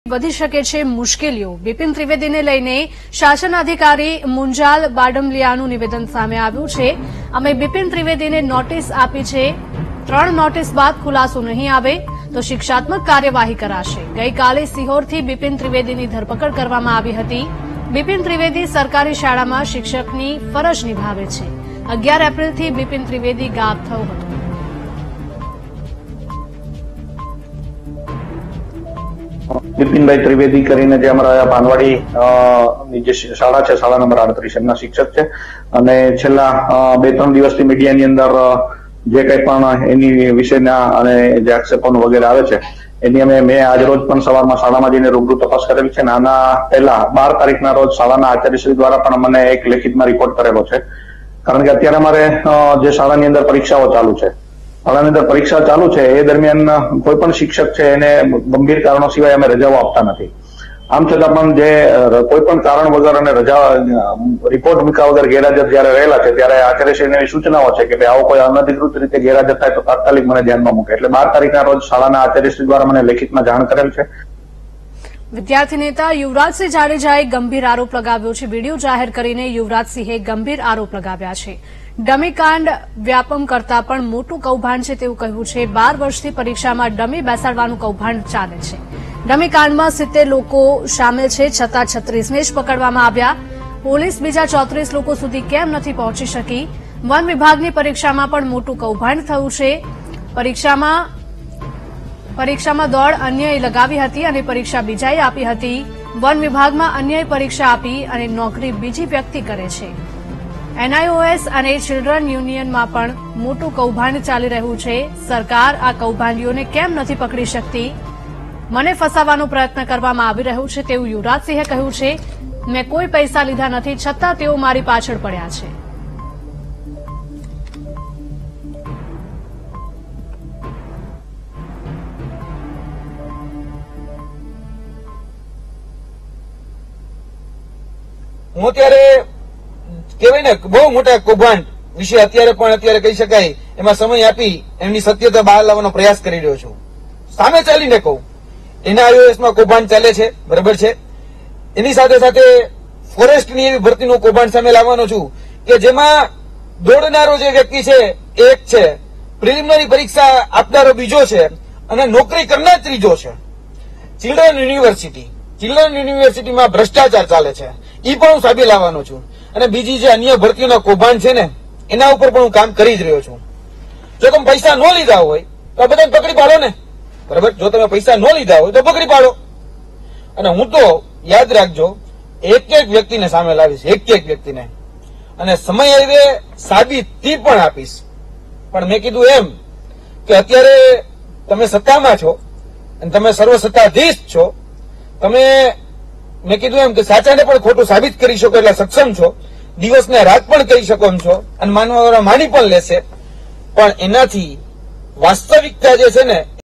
Bhavnagar ડમીકાંડ મામલે બિપીન ત્રિવેદીની વધી શકે મુશ્કેલી,સિહોરથી પકડ બિપિનને ભાવનગર લવાયો
બિપીન ત્રિવેદીને લઈ શાસનાધિકારી મુંજાલ બડમલીયાએ આપ્યું નિવેદન,'અમે બિપીન ત્રિવેદીને નોટિસ આપી છે, 3 નોટિસ બાદ ખુલાસો નહીં આવે તો શિક્ષાત્મક કાર્યવાહી કરાશે | VTV Gujarati